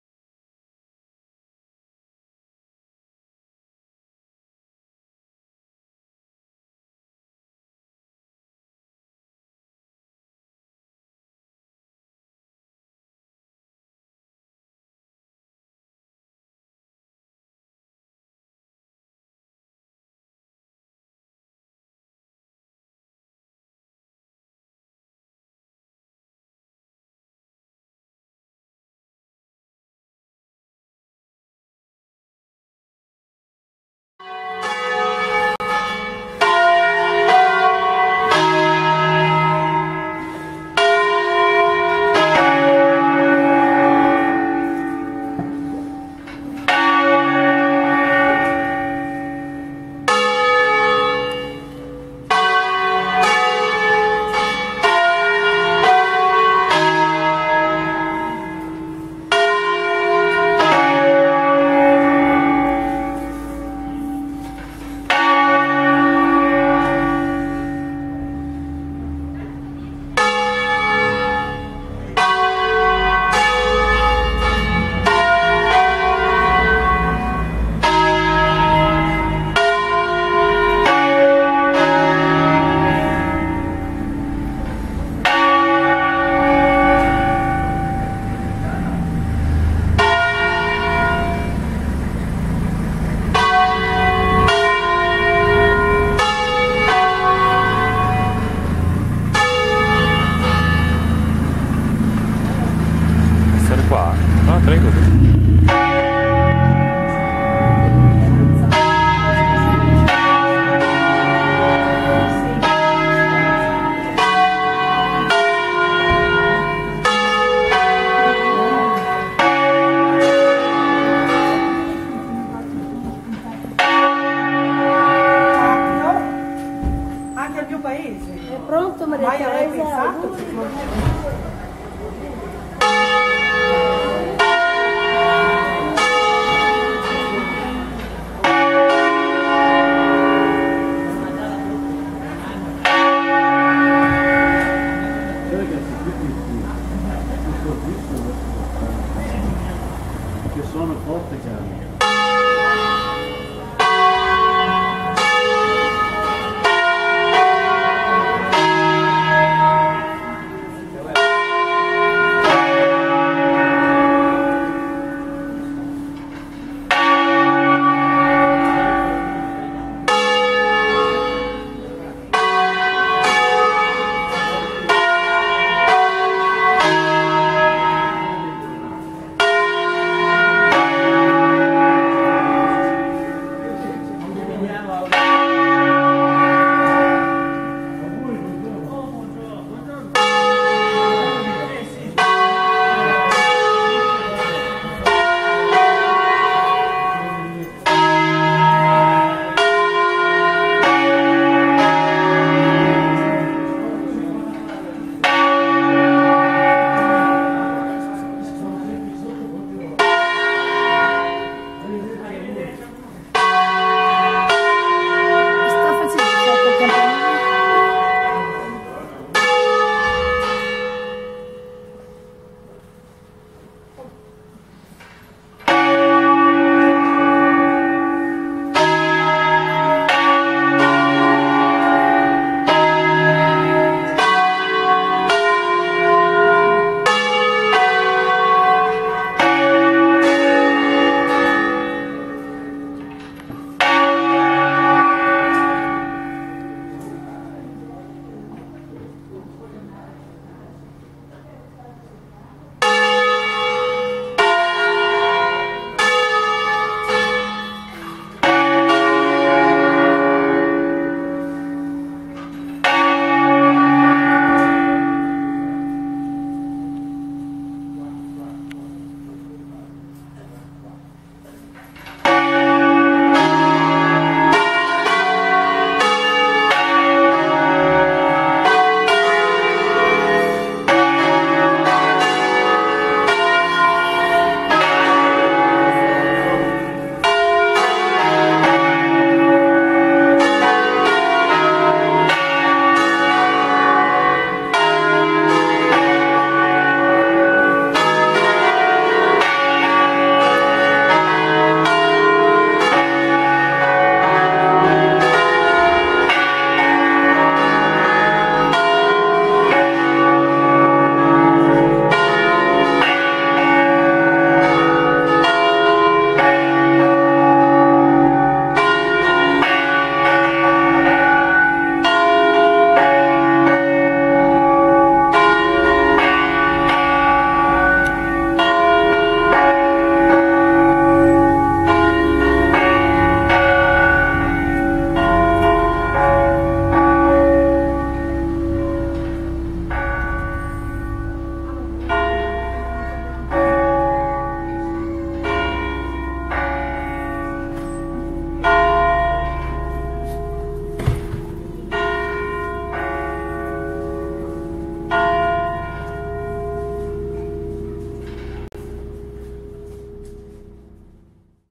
Le campane di Barzio (LC)
Concerto di 5 campane in Reb3
Bizzozero 1856 (fuse a Milano)
Concerto solenne a 5 campane per la S. Messa delle h. 10.30 nella solennità del Natale di Nostro Signore.

Sul campanile è presente anche un sonello in La4 fuso da Bonavilla nel 1706